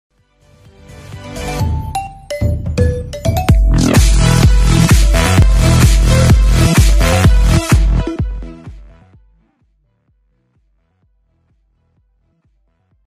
MalloDK Intro - By Daniel
Yezz c: Her er en intro til MalloDK :)
Jeg er selv ret tilfreds med resultatet :)
Hvad syntes i? :) Kom endelig gerne med feedback, det  etyder utroligt meget! :)

PEACE!
//BirdieGraphiX